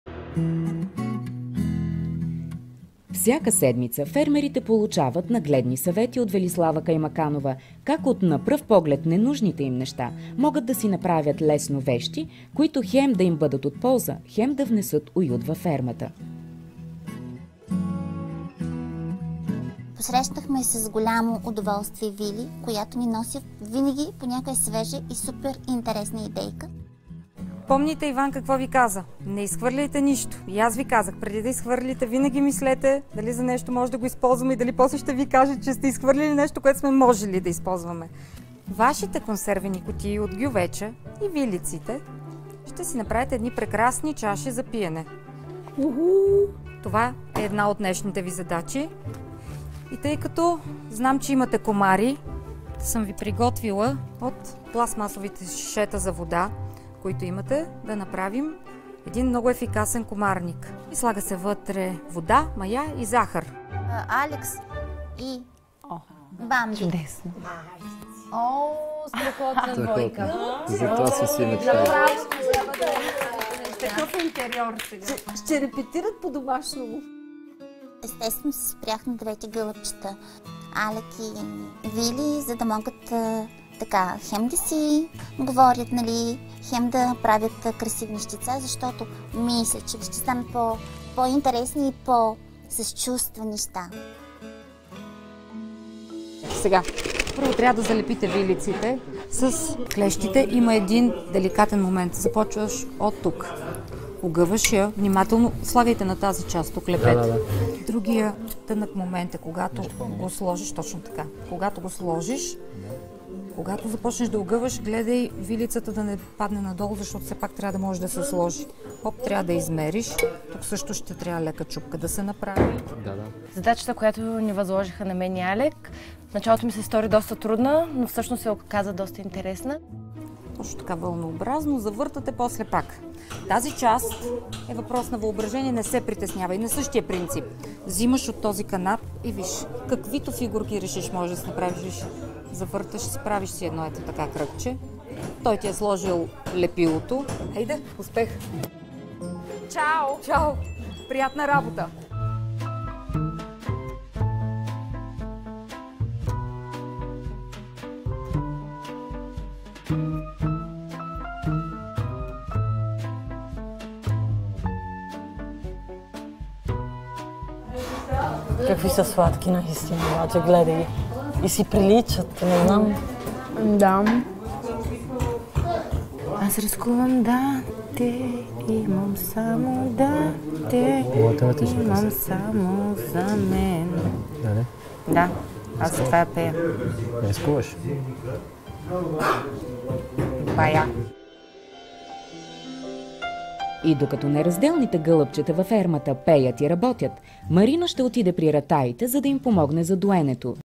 Велислава Каймаканова - Как от ненужни неща да си направим необходими вещи
Велислава Каймаканова - Ножарова и нейните съвети как да направим от нищо - нещо или вещите които не използваме повече, да влезнат в употреба отново. Изгледайте цялото видео за Фермата - част 2.